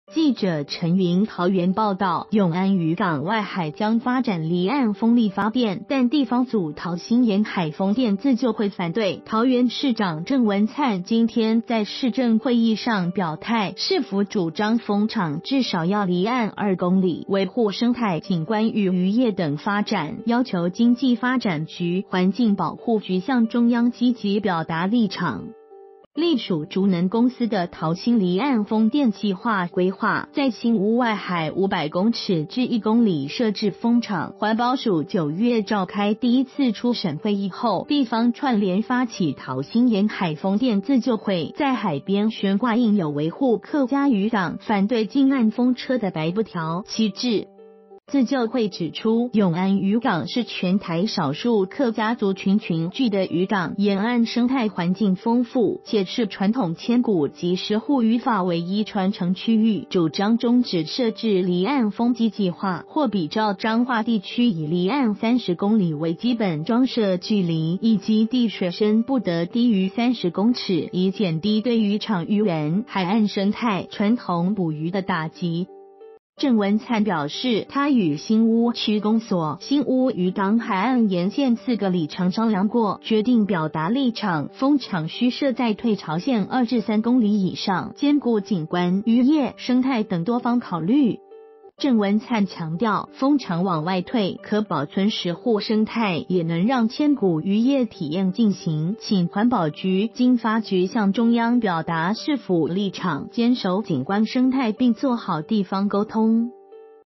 永安漁港發展離岸風電 鄭文燦：至少離岸2公里
［記者陳昀／桃園報導］永安漁港外海將發展離岸風力發電，但地方組「桃新沿海風電自救會」反對，桃園市長鄭文燦今天在市政會議上表態，市府主張風場至少要離岸2公里，維護生態景觀與漁業等發展，要求經濟發展局、環境保護局向中央積極表達立場。